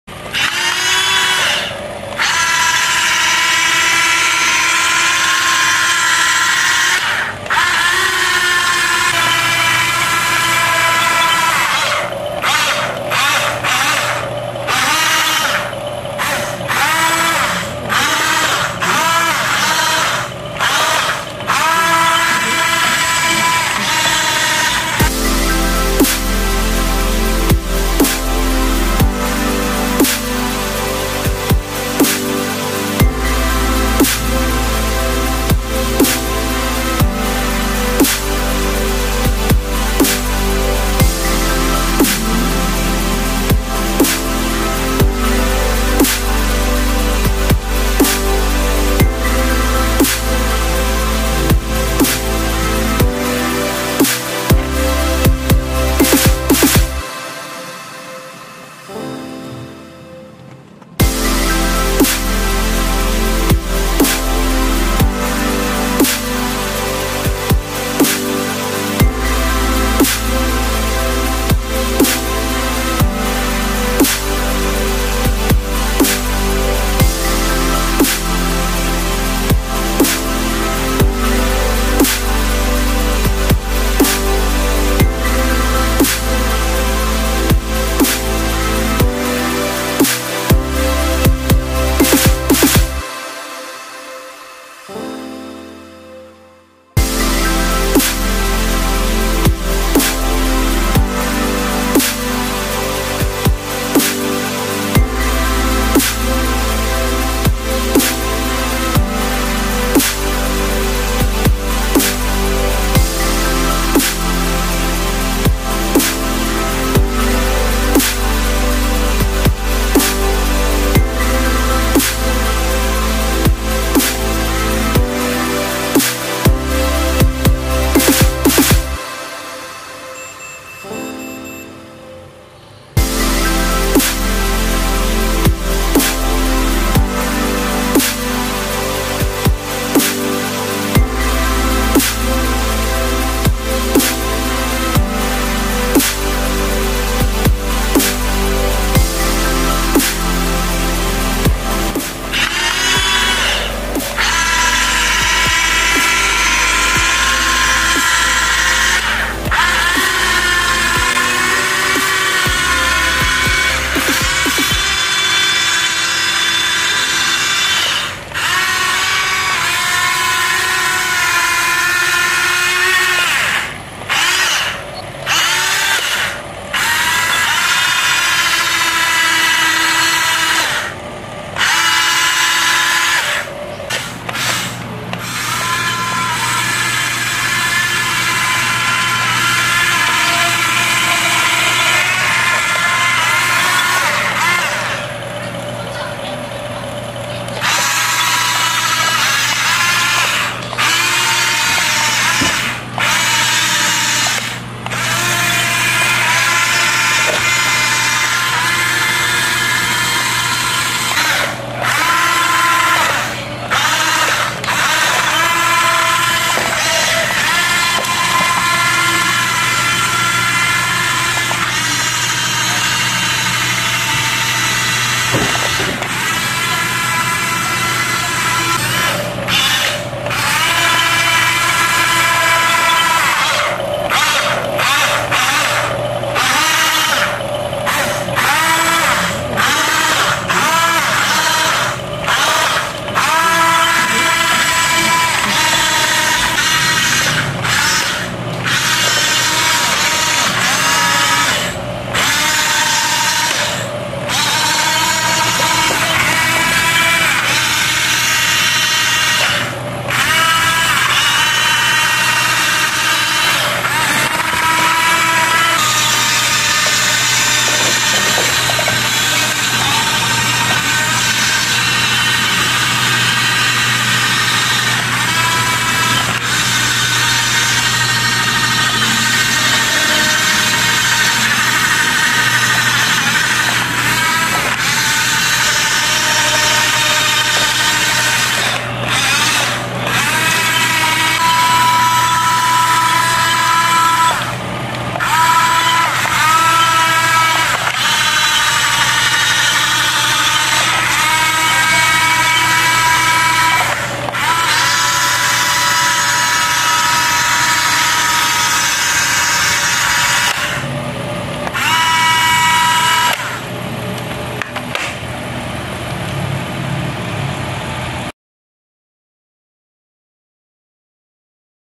Huina 1550 modified making crawler | rc excavator huina 1550
just for fun
modified rc excavator huina 1550 with rc toys crawler